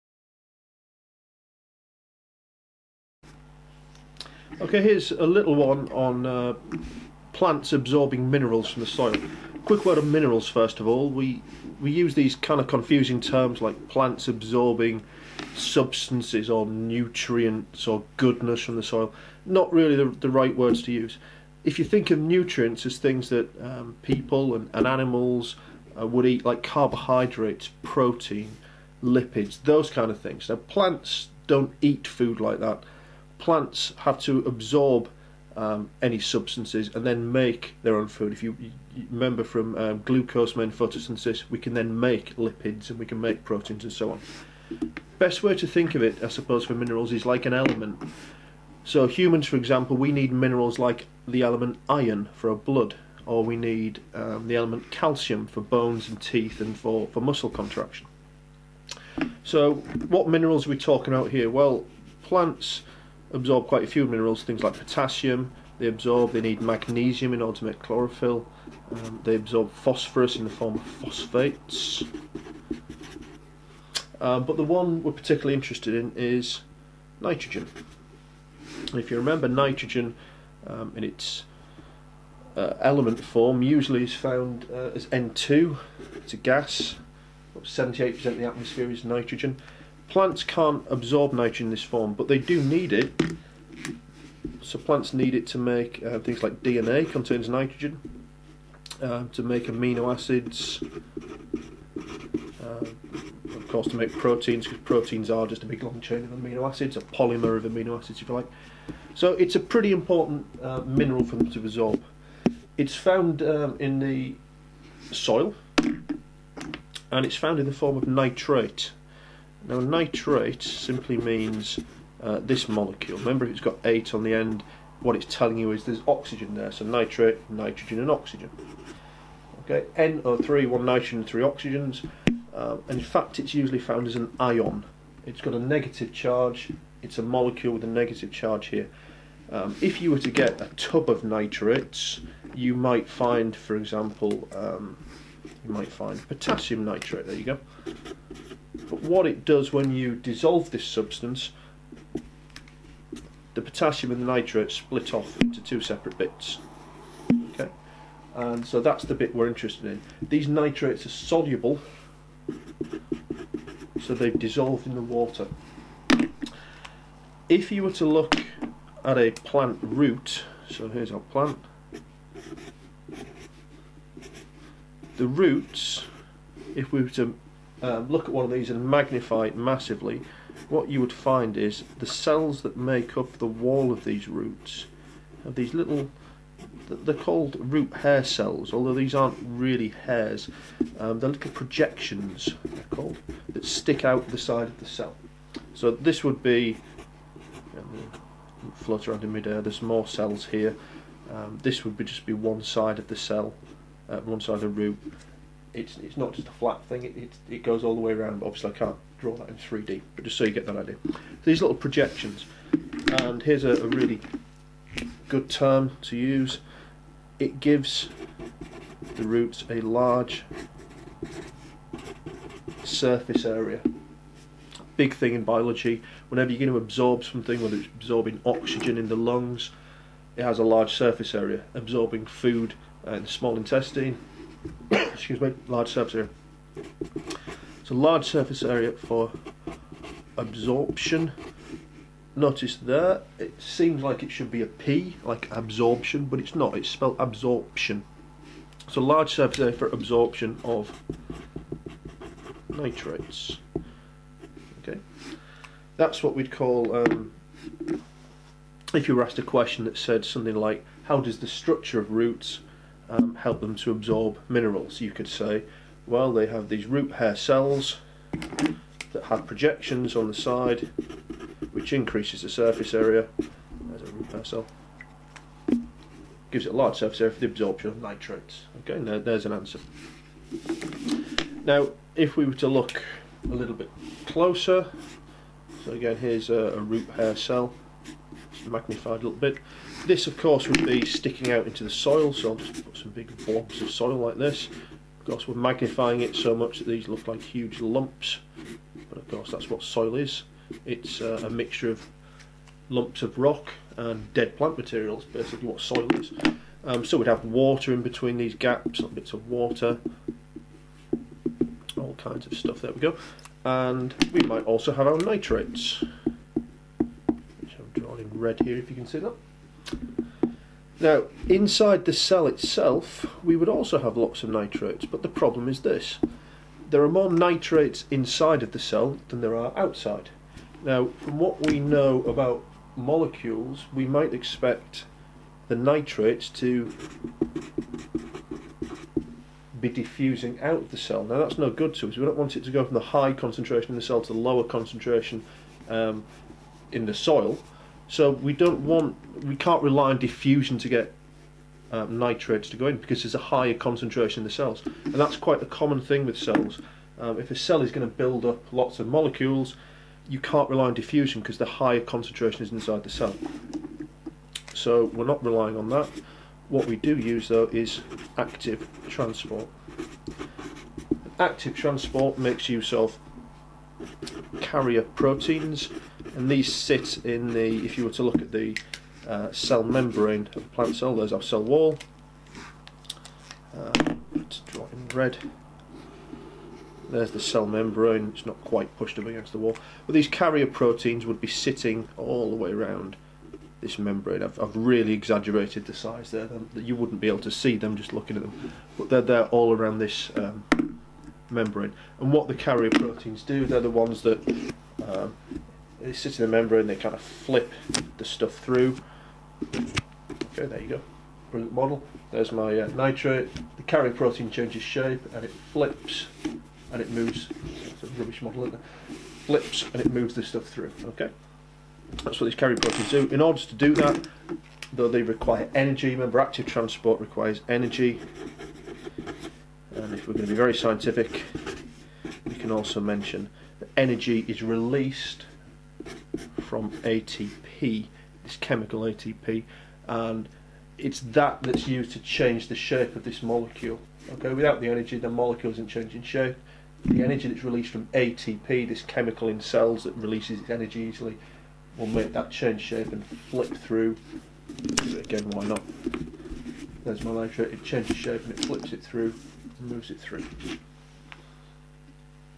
B4 Minerals in the soil
B4 GCSE OCR 21st century science - Howdo plants absorb minerals